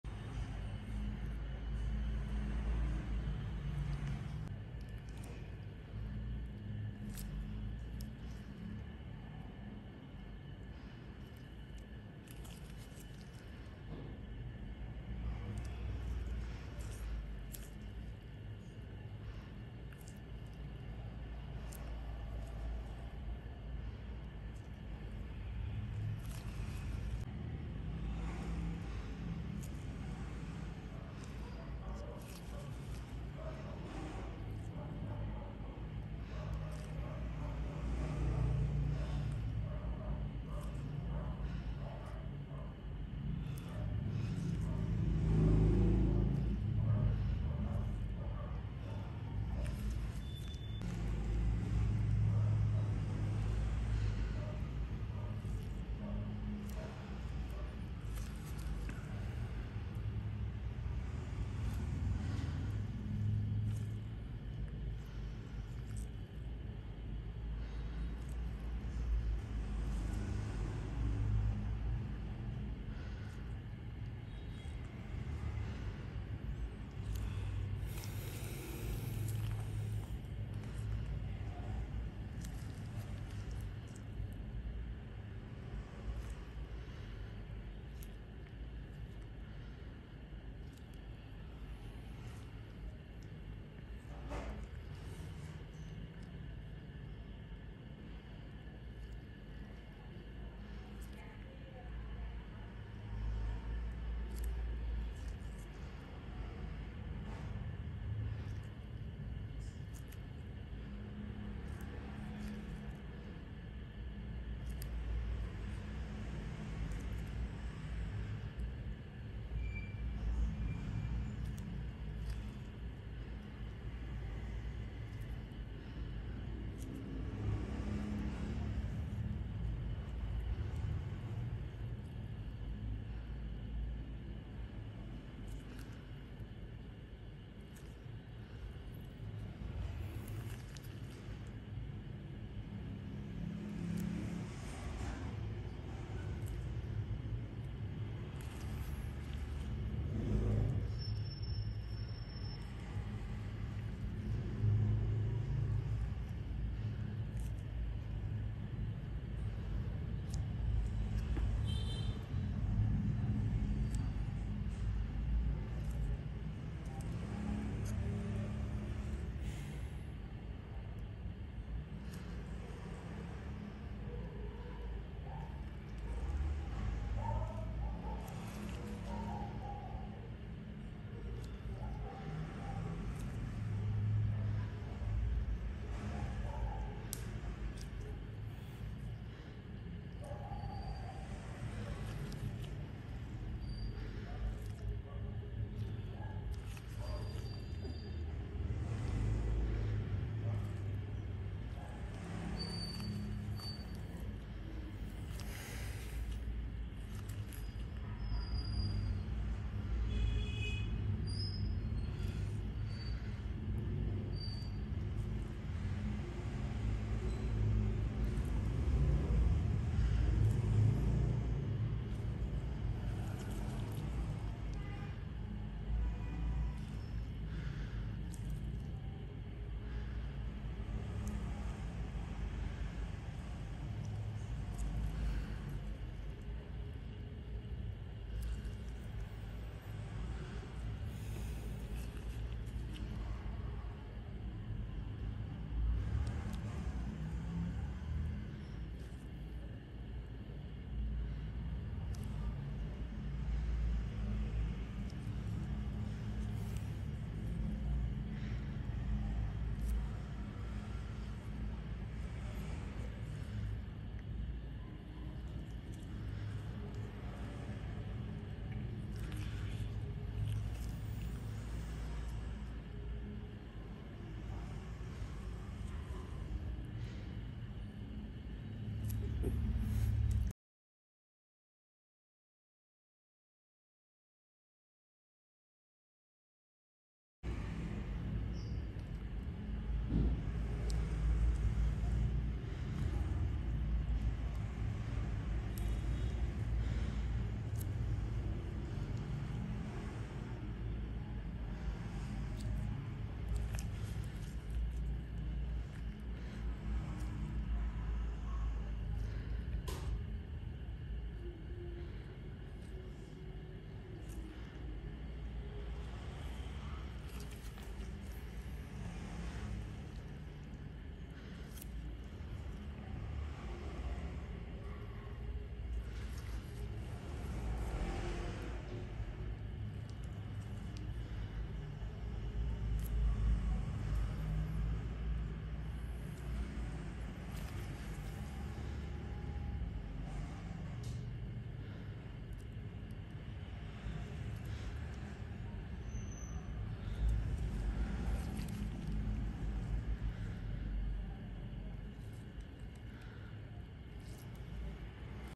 Relaxing Acne Treatment Everyday With Vu Quynh Mi Spa|Blackheads Removal| Cystic Removal 2023 |#036
I work hard every day to share relaxing moments with you through daily squeezing videos.
Relax everyday with Vu Quynh Mi Spa